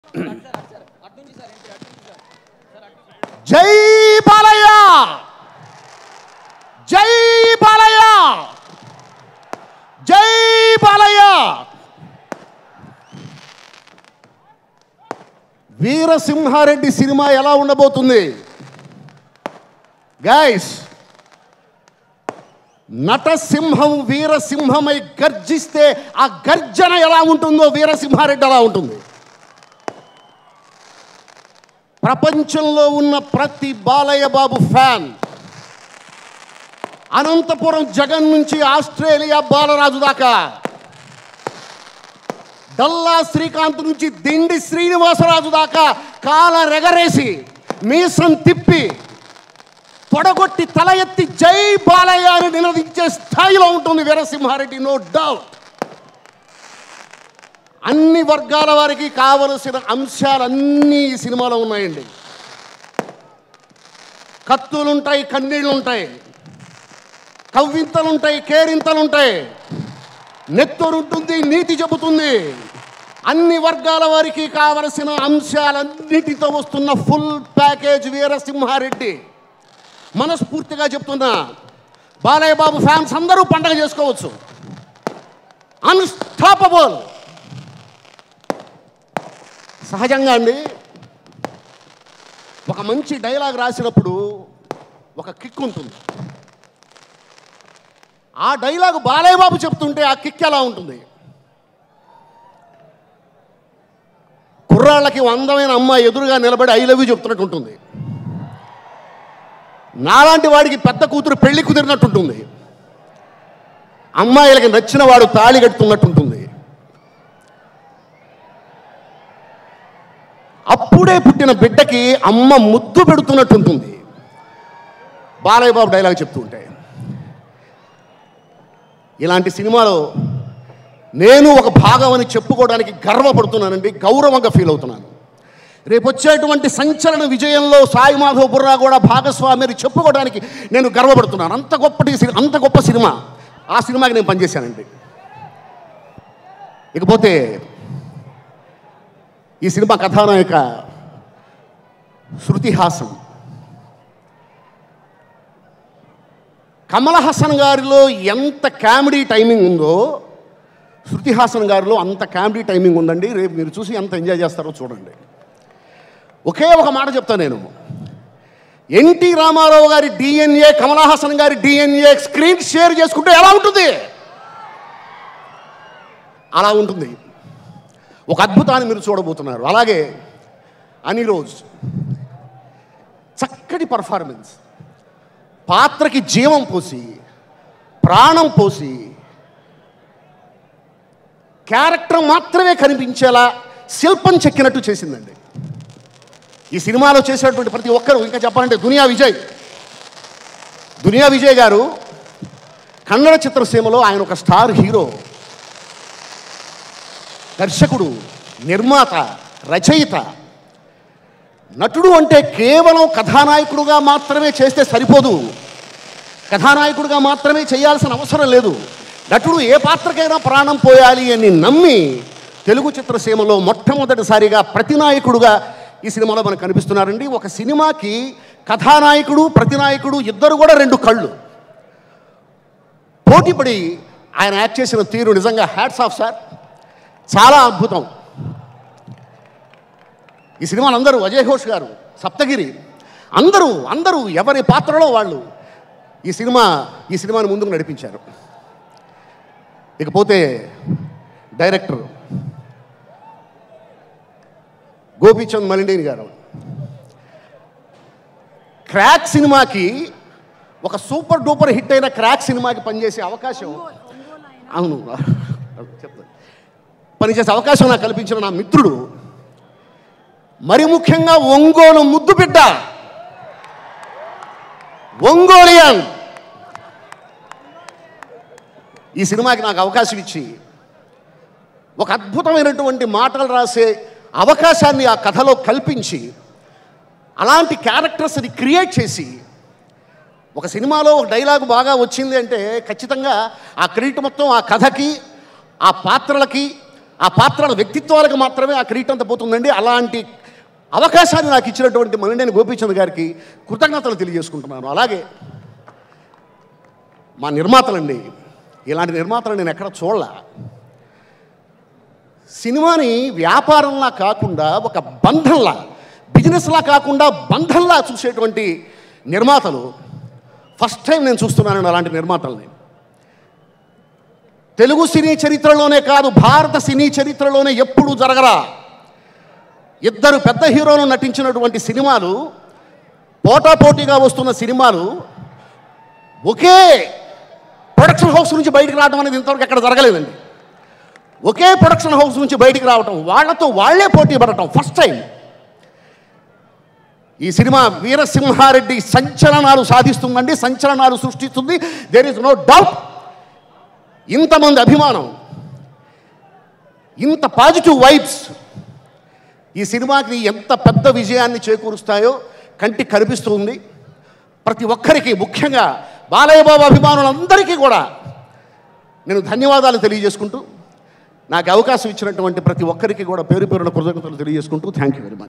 Sai Madhav Burra Speech At Veera Simha Reddy Pre Release Event Nandamuri Balakrishna
Dialogue Writer Sai Madhav Burra Speech At Veera Simha Reddy Pre Release Event Video